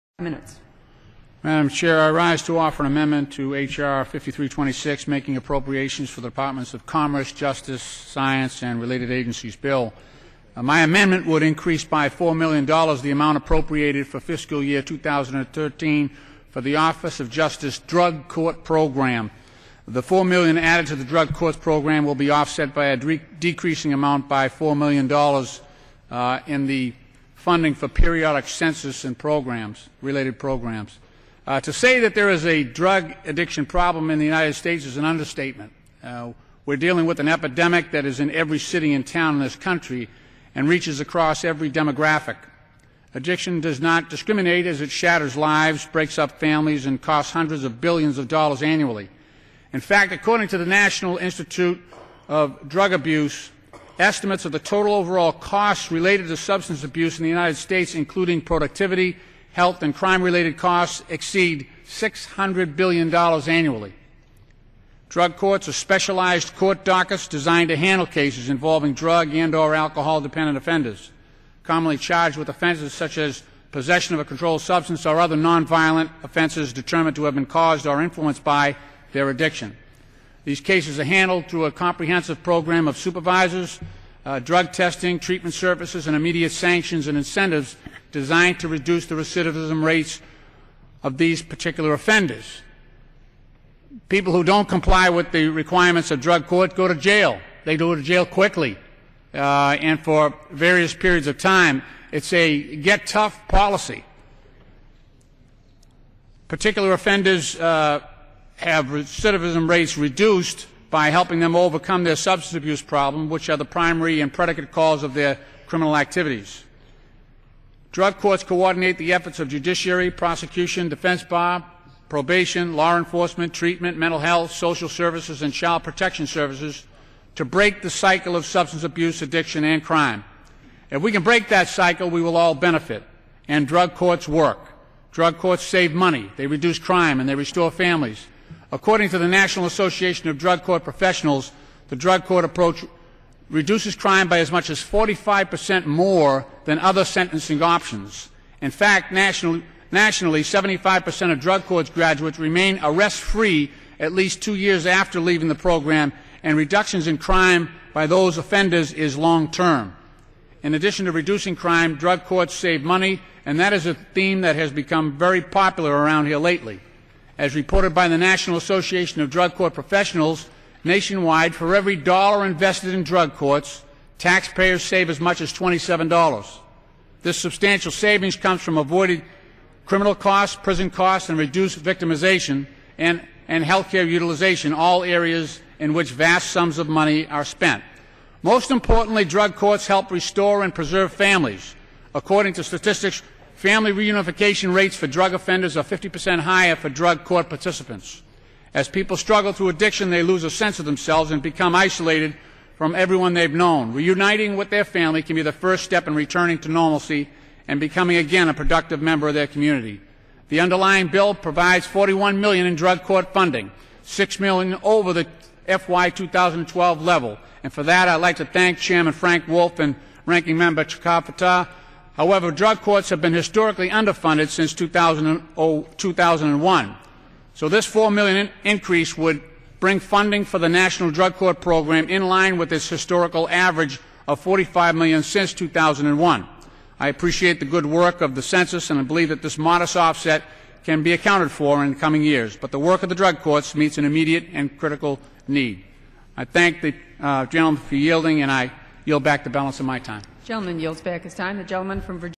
Floor Speech on Increasing Drug Court Funding
The House of Representatives approved an amendment on Tuesday, May 8th, offered by Congressman Lynch that would increase drug court funding by $ 4 million.  The amendment will be included in H.R. 5326, the FY2013 funding bill for the Departments of Commerce, Justice, Science, and Related Agencies.